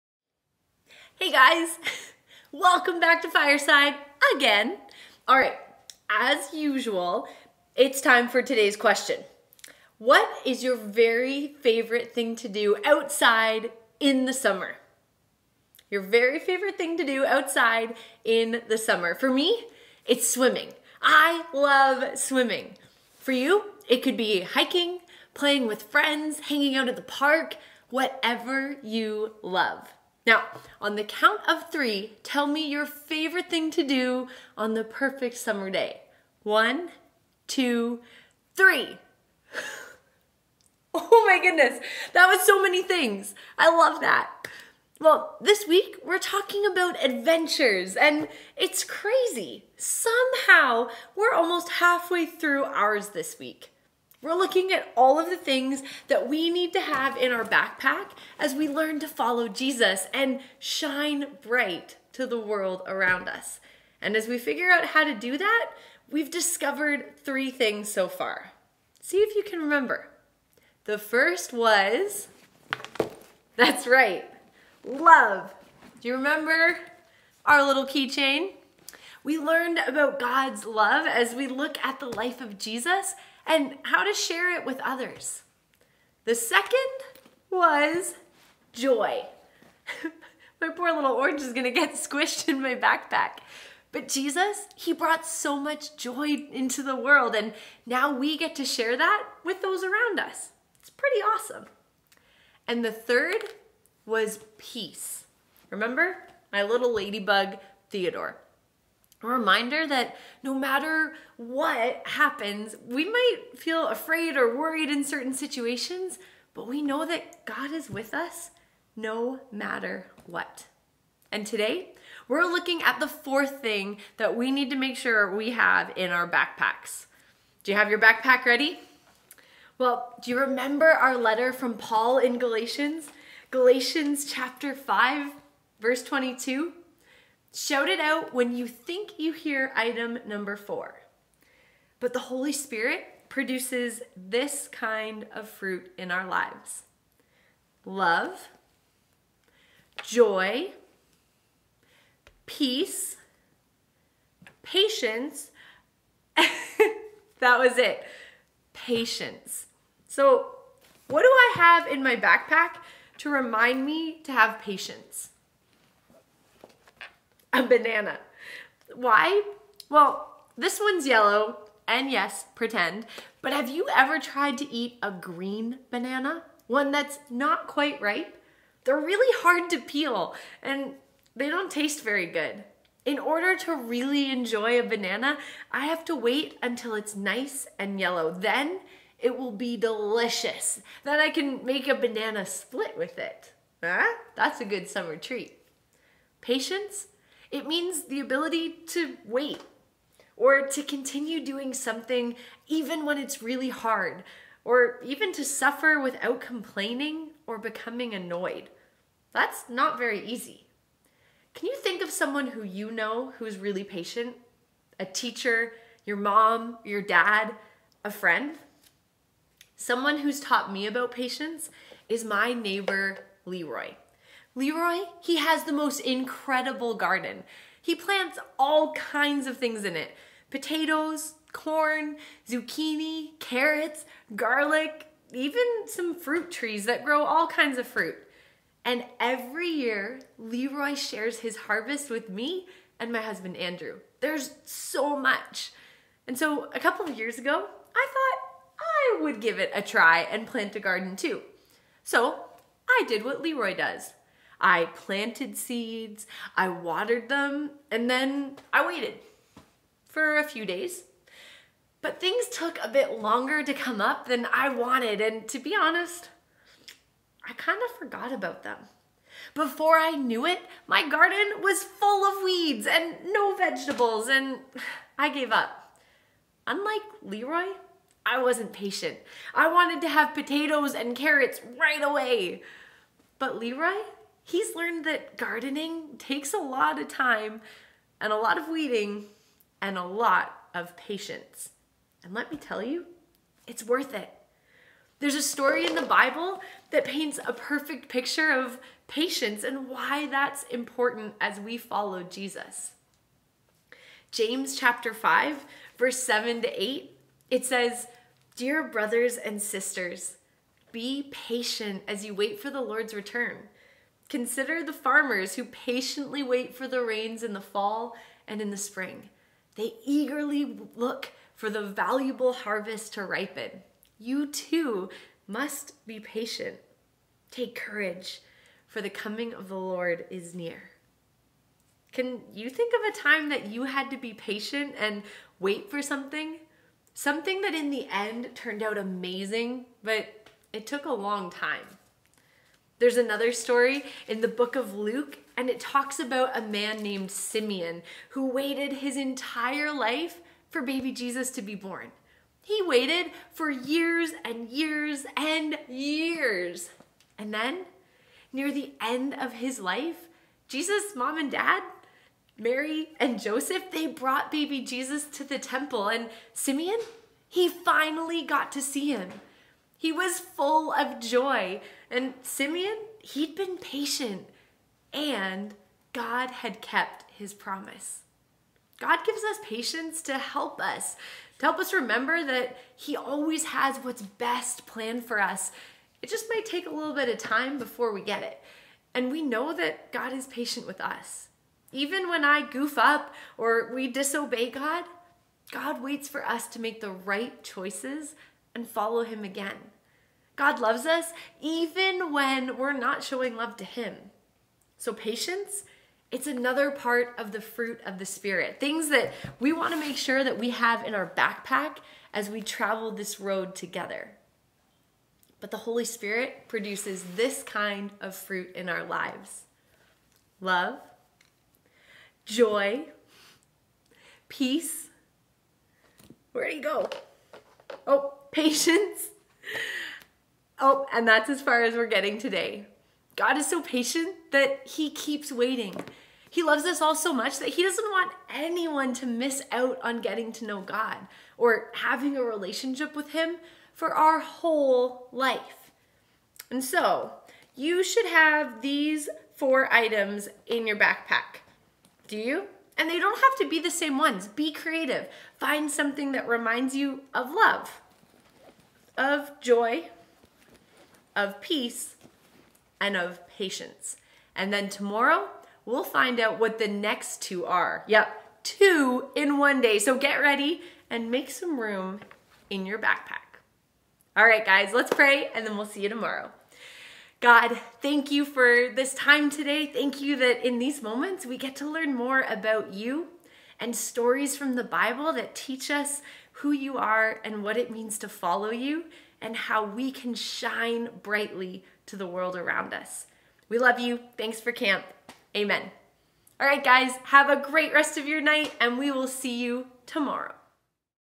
Blast (Tuesday) Camp Speaker
It's time again to hear from Sarah!

We hope you enjoyed your activity today. Two activities down and three to go! There's still lots of fun in store!